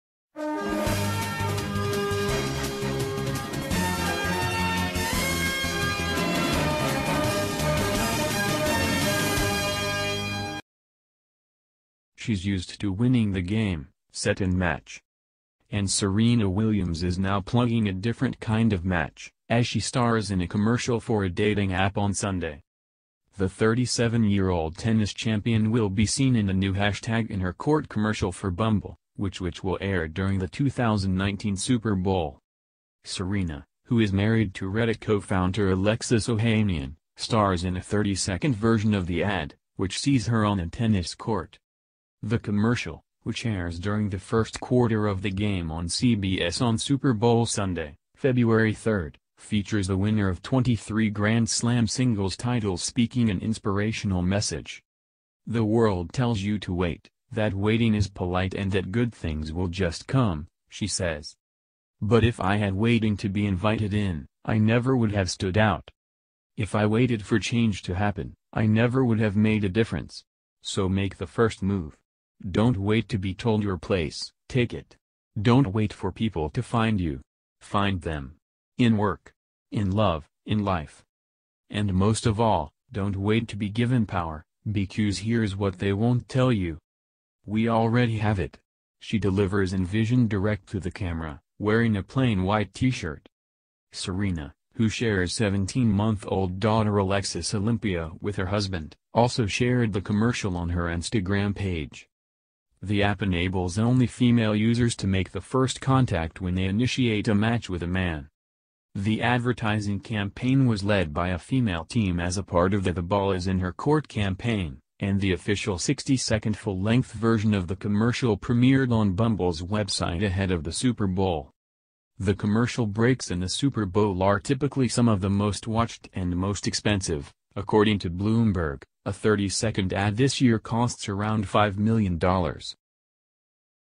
Serena William shares empowering message as she stars in Super Bowl commercial for dating app Bumble
Serena Williams shares empowering message as she stars in Super Bowl commercial for dating app Bumble